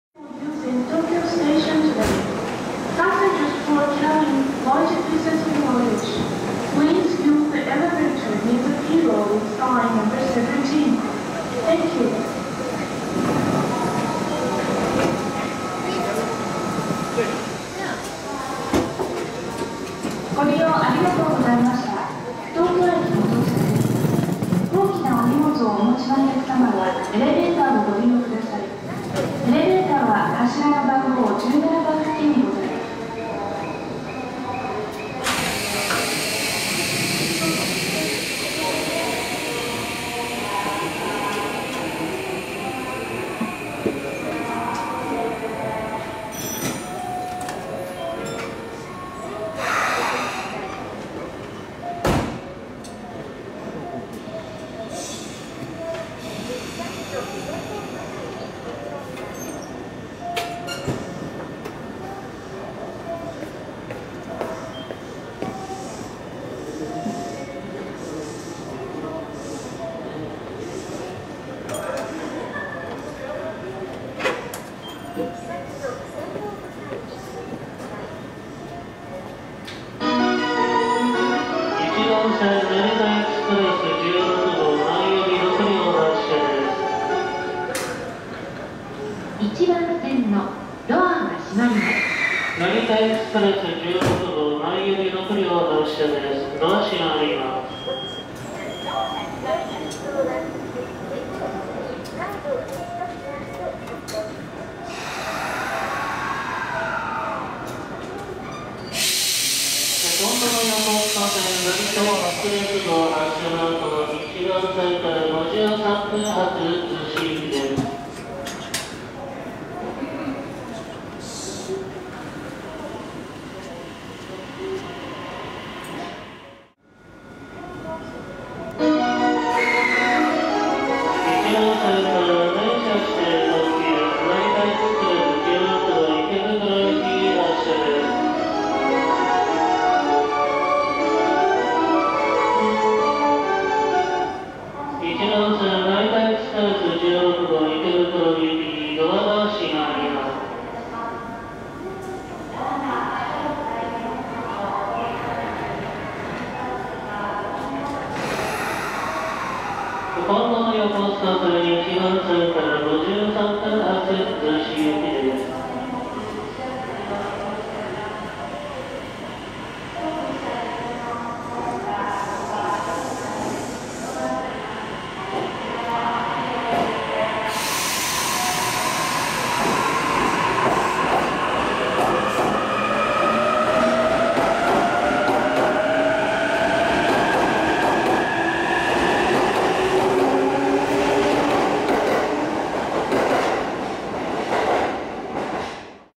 JR東日本E259系 特急成田エクスプレス16号 東京駅 発車（分割シーン有）
撮影日2012/12/30
東京駅を発車するところです